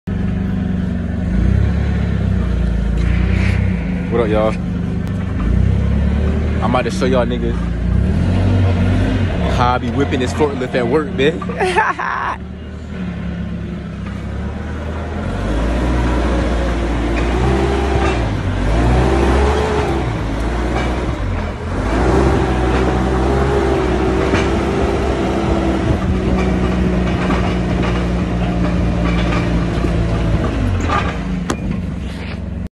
Riding around the Fedex Freight dock (Freight Handler)
Just a quick video of the fedex freight loading dock. I couldn’t get a lot of shots because I didn’t want to get fired